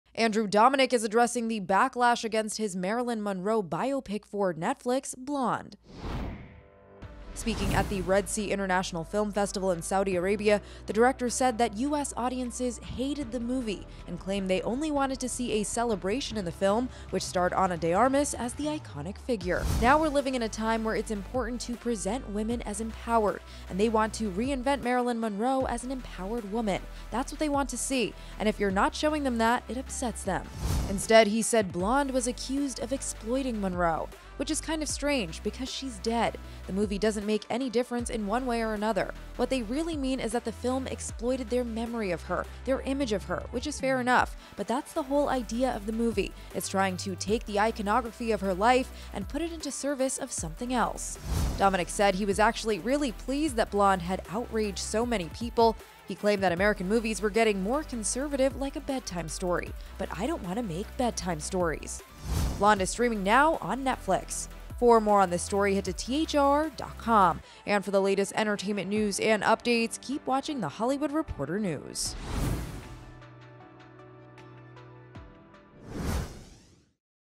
Andrew Dominik Fires Back On ‘Blonde’ Backlash That He's Exploiting Marilyn Monroe | THR News
The director also said he was "really pleased" the film starring Ana de Armas had "offended so many people."